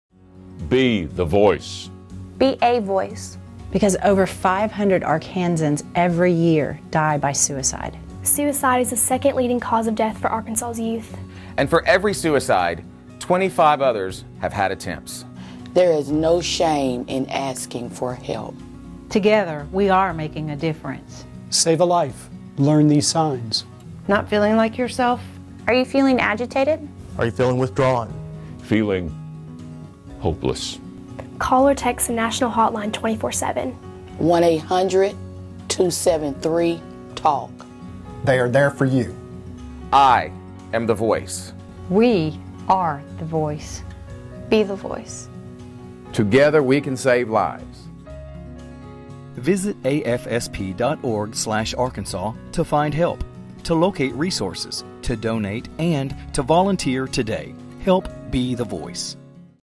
SUICIDE PREVENTION 60 SEC PSA 480
September is Suicide Prevention Awareness month.
The Arkansas chapter of the American Foundation for Suicide Prevention collaborated with citizens to promote the Be The Voice campaign to help stop suicide.